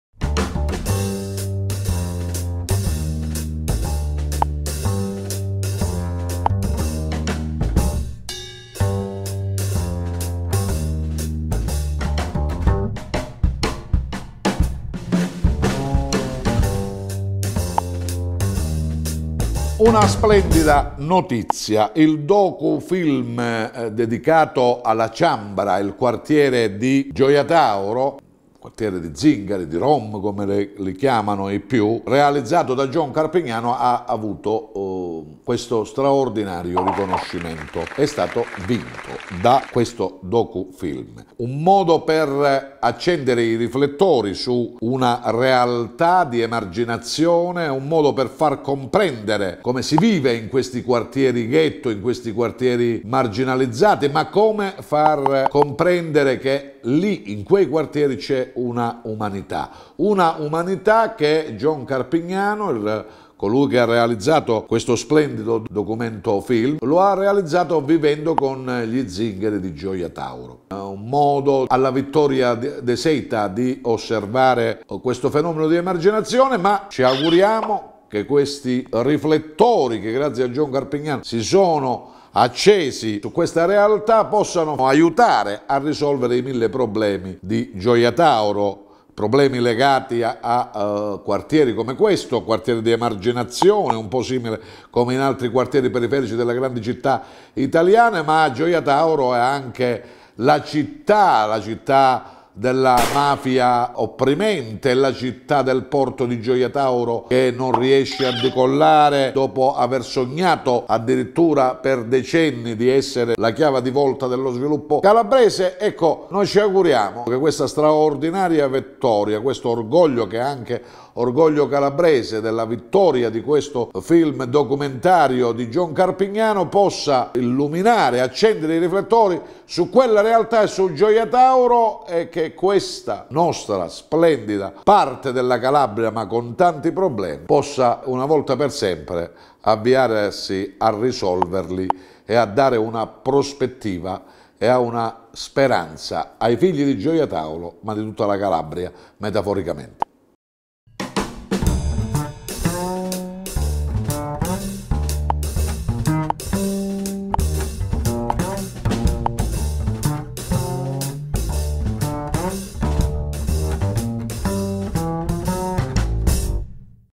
Il WhatsApp di...? - Pasquale Motta - 25-03-2018
IL WHATSAPP DI... Pasquale Motta [Direttore Responsabile LaCnews24.it] - Jonas Carpignano ha vinto il David di Donatello per la regia del film "a Ciambra",  docu-film ambientato nell'omonimo quartiere-ghetto, abitato dai Rom di Gioia Tauro. Questo premio serva da denuncia e da speranza.